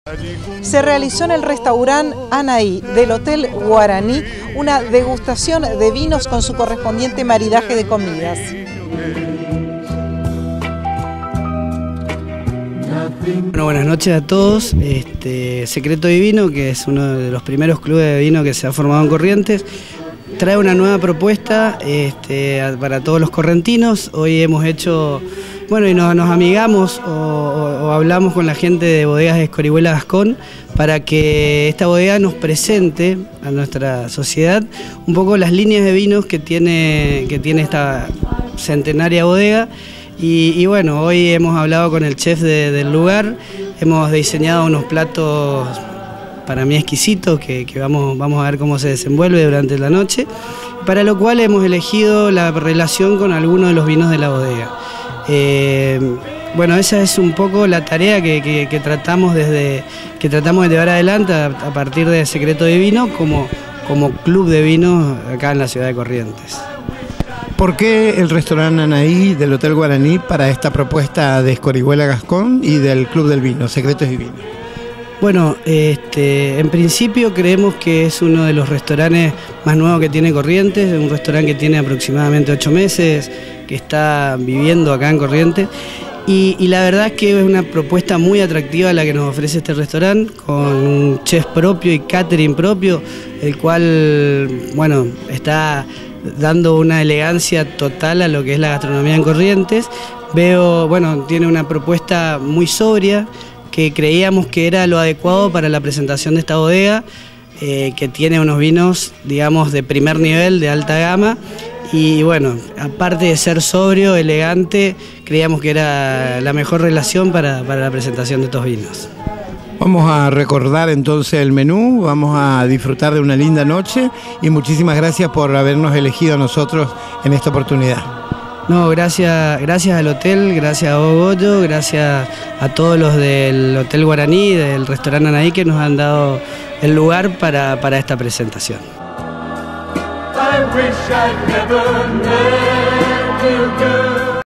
Degustación de Vinos
Realizada en el Hotel Guaraní, agradecemos a Goyo Prieto por la invitación . . .